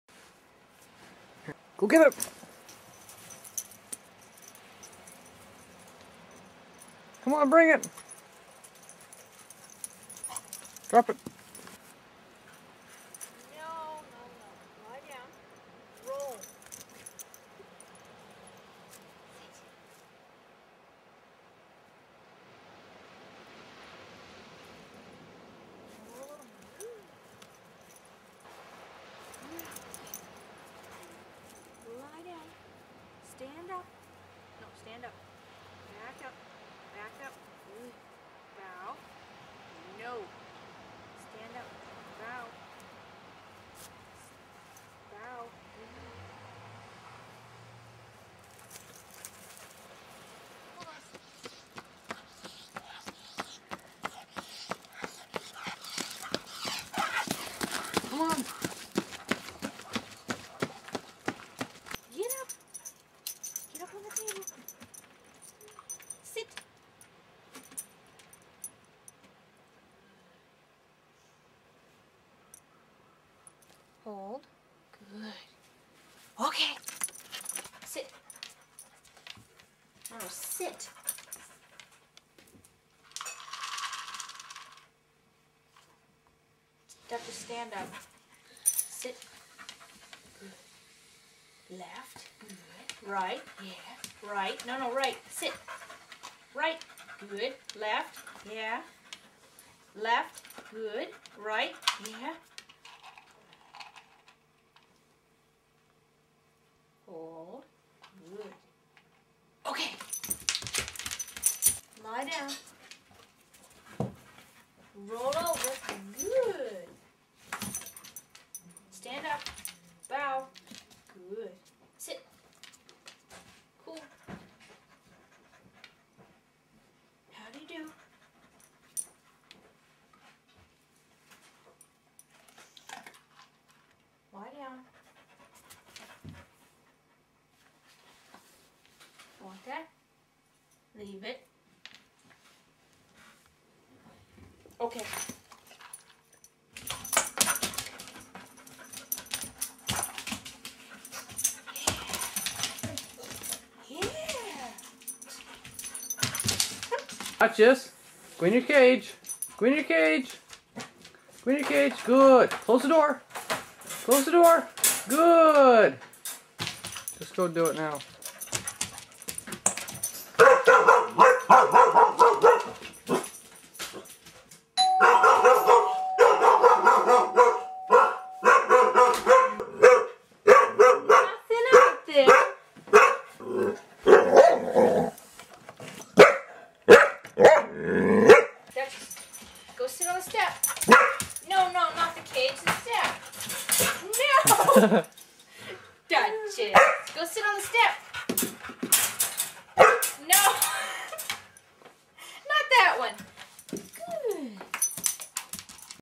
Duchess The Border Collie
Duchess the 1 year old Border Collie in action.